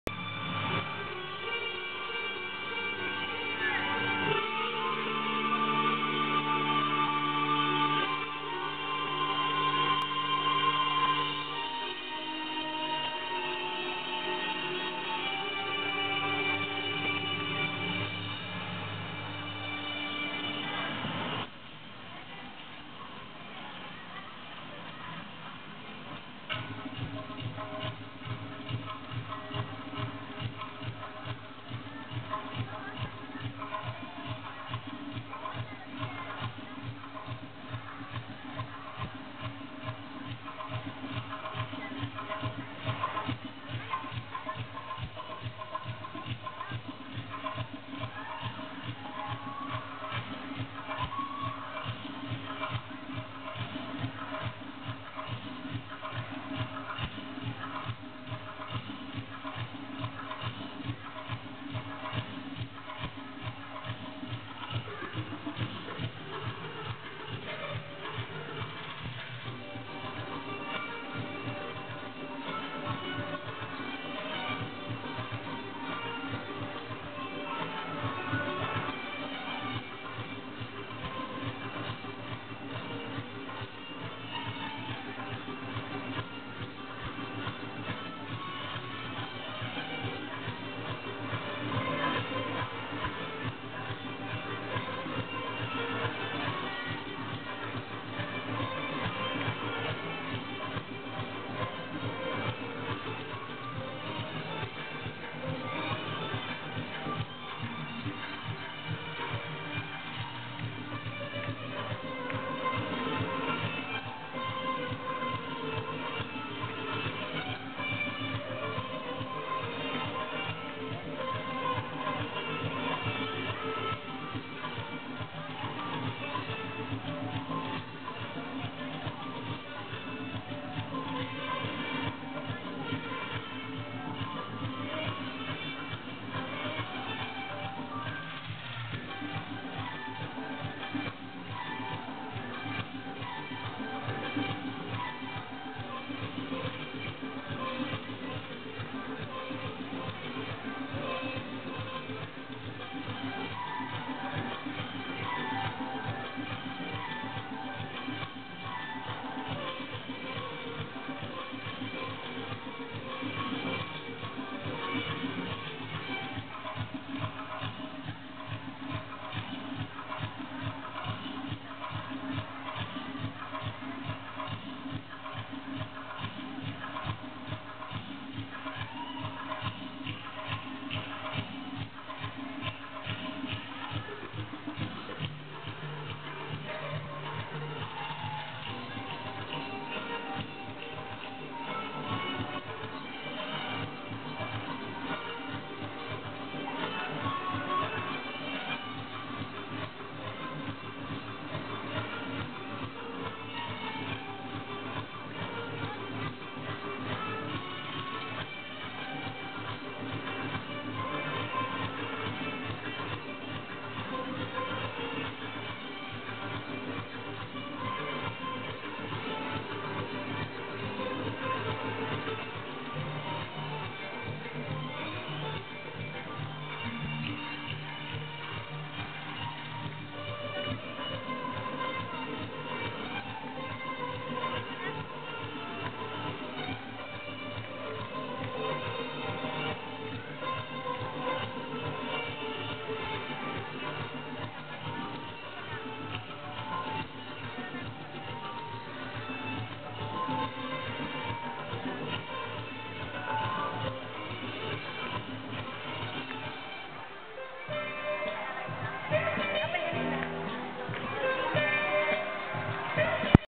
Brent's Persian Dance
Graduation Program: Superheroes visiting the world to save mother earth. Brent was assigned as a Persian dancer.